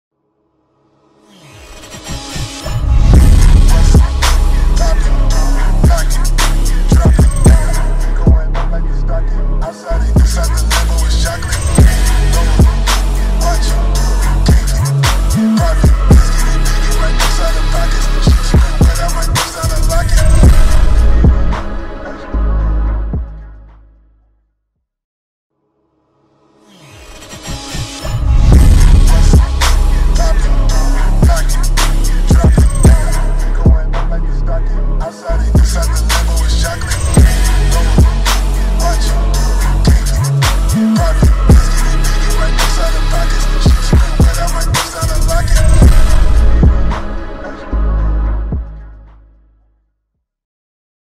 supreme | virel x surve [c4d]
experimenting a bit over here, what you think?

x song: ask surve
x font: idk don't ask
x lightroom and materials: guess who to ask
x discord: virel#0305

0:00 - 0:25 c4d: surve, ae: surve
0:25 - 0:51 c4d: surve, ae: virel